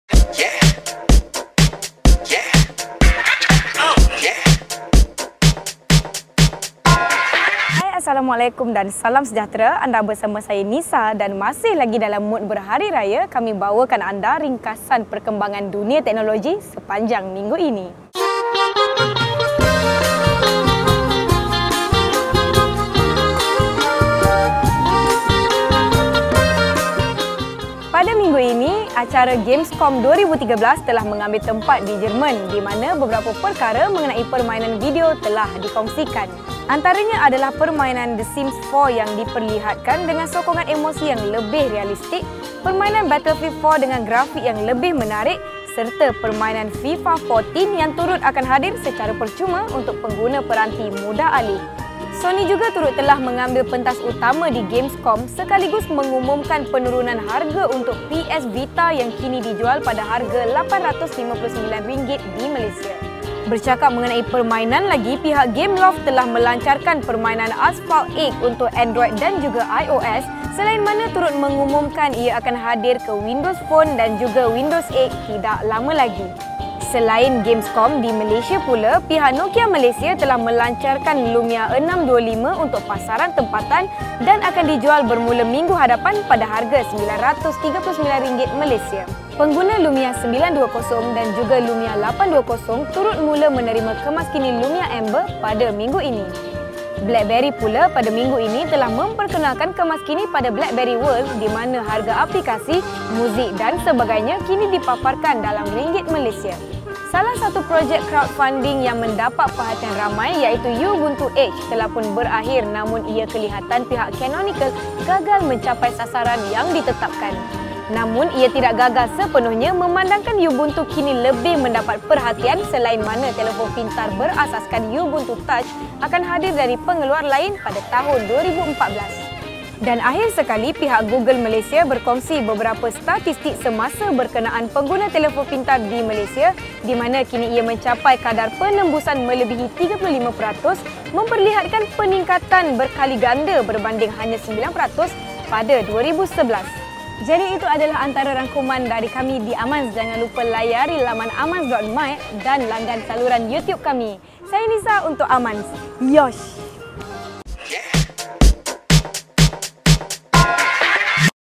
Mingguan Amanz - Lumia 625, Ubuntu Edge Tamat, Gamescom
Pada minggu ini, Gamescom 2013 telah mengambil pentas utama, dimana Sony dan Microsoft masing-masing mengumumkan permainan untuk konsol mereka, termasuk EA yang turut berkongsi beberapa permainan baru daripada mereka/

Di Malaysia pula, Nokia Malaysia telah mengumumkan Lumia 625, serta mula memperkenalkan kemaskini Amber kepada pengguna Lumia 920 dan 820 di Malaysia.

Pada minggu ini juga, pengumpulan dana untuk Ubuntu Edge ditamatkan, dan mereka hanya mencapai hampir $13 juta sahaja melalui IndieGogo.